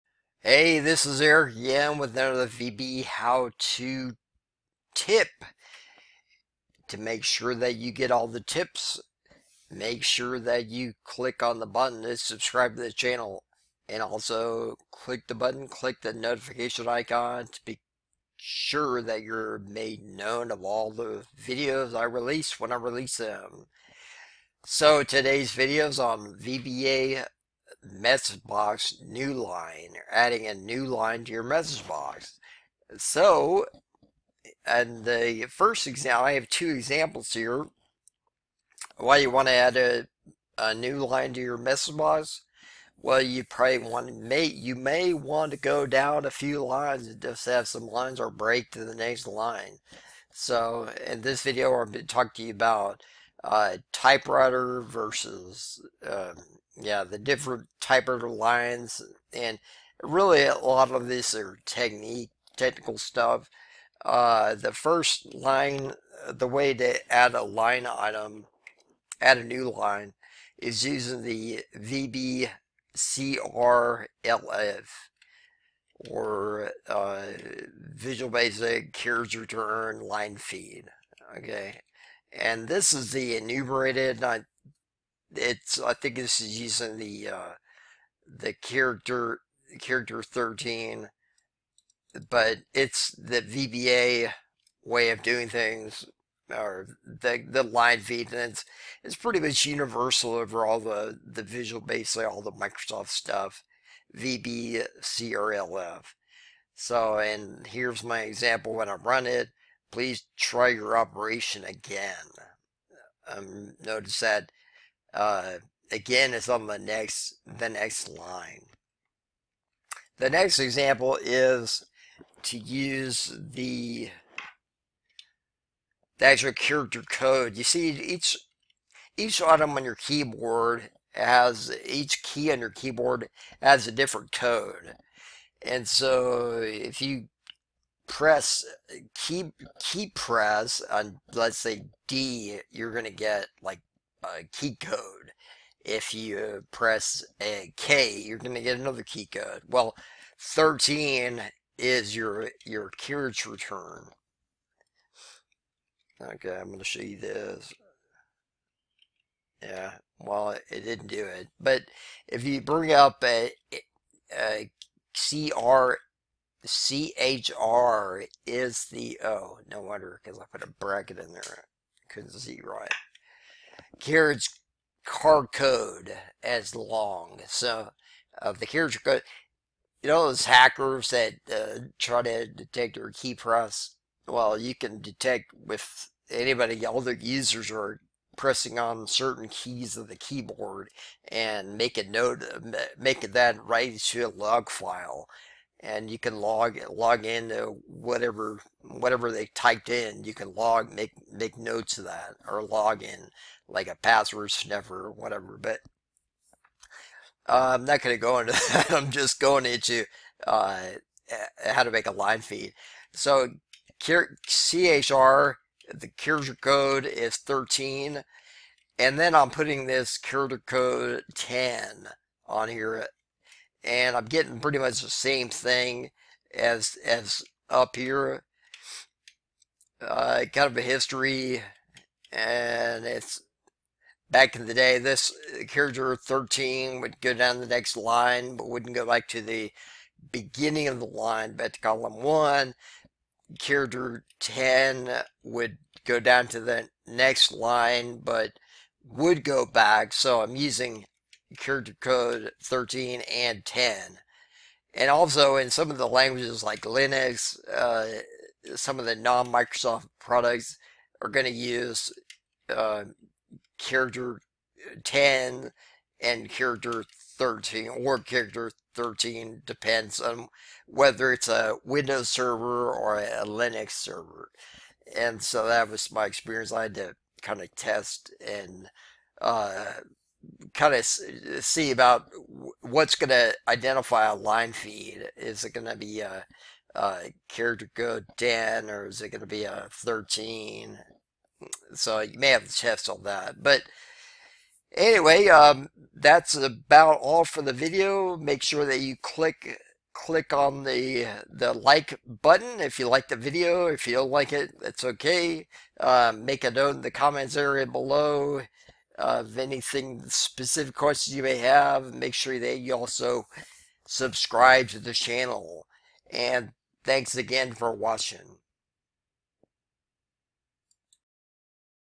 A VBA Msgbox New Line Example
This video will show you A VBA Msgbox New Line using character codes to your vba message box. Get the code at: